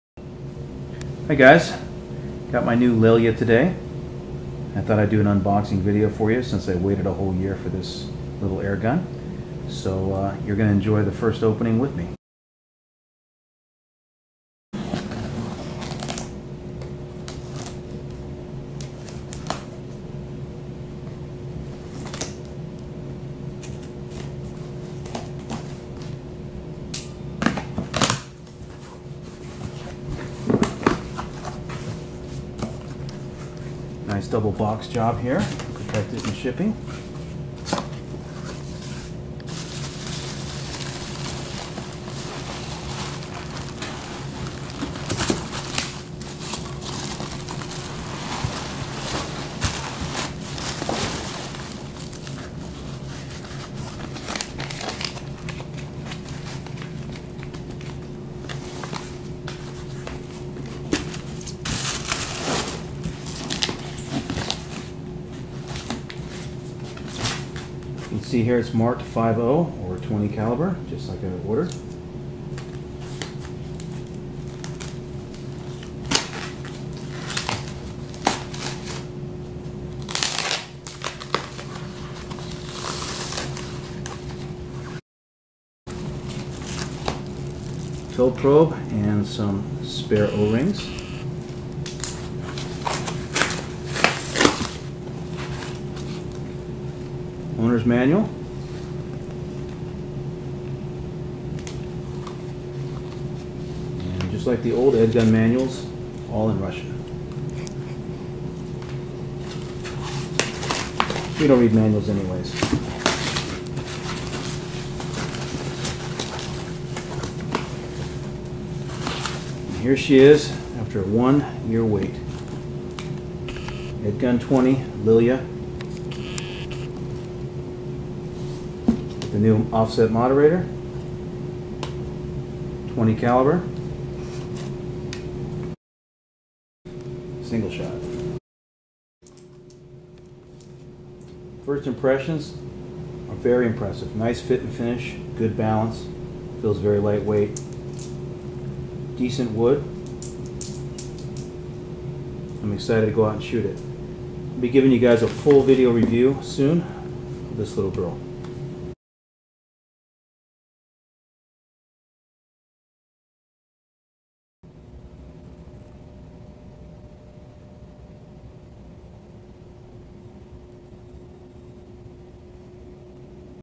EDgun Lelya .20 unboxing
Opening my EDgun Lelya for the first time. A full review is coming soon